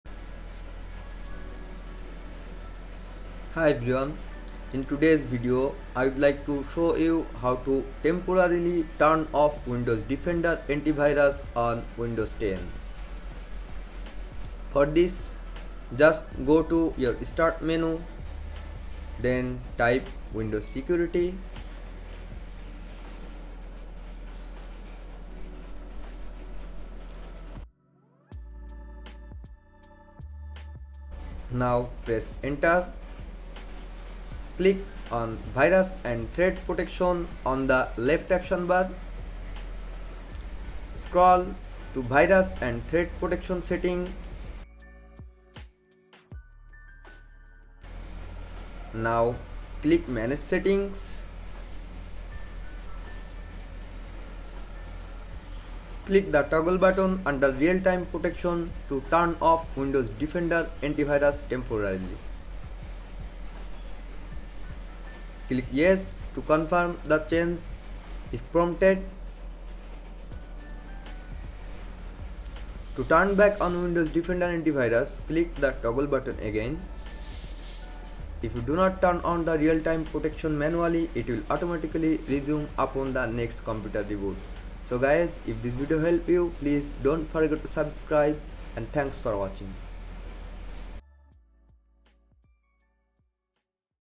How to Disable Antivirus on Windows 10 UPDATED | Turn Off Windows Defender in Windows 10
Learn How to Disable Antivirus on Windows 10. You can easily turn off windows defender and antivirus on windows 10 by simply following this tutorial.